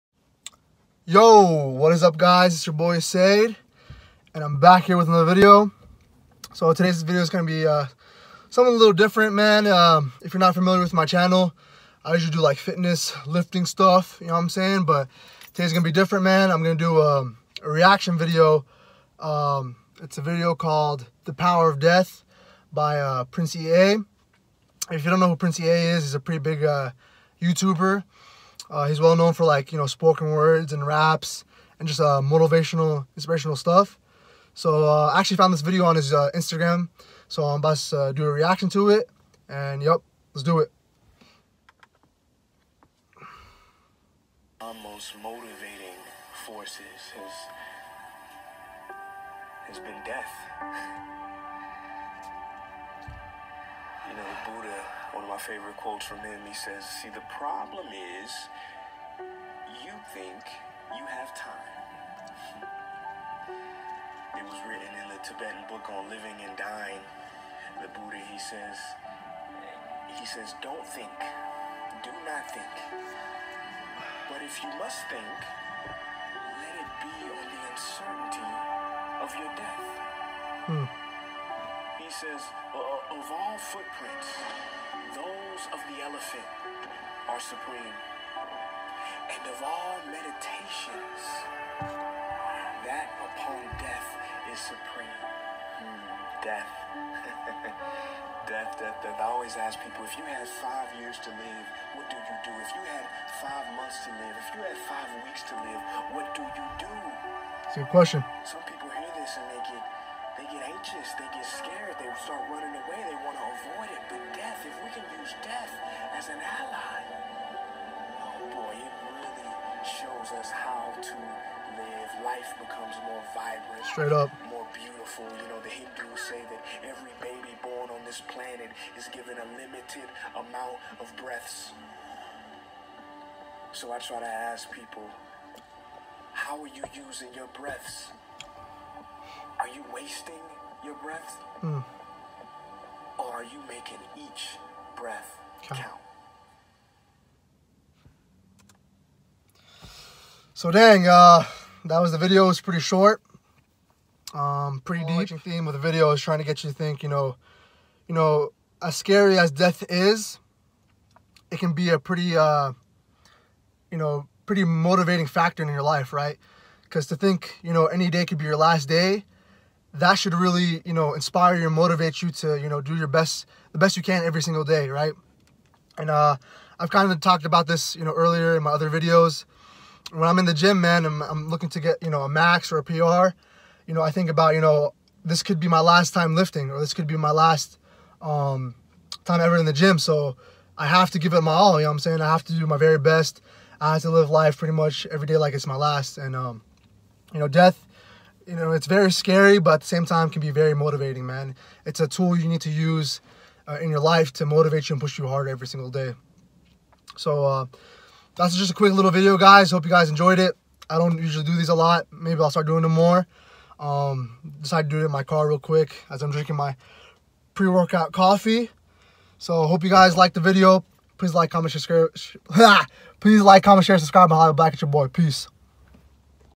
THE POWER OF DEATH - by Prince Ea - REACTION powerful message!
All praise and Glory be to God
This is my first ever reaction video, and what better video to do that with. I talk about importance of death and how it can be used as a tool to inspire you a lot on my channel. So I thought I would react to what Prince ea had to at about it!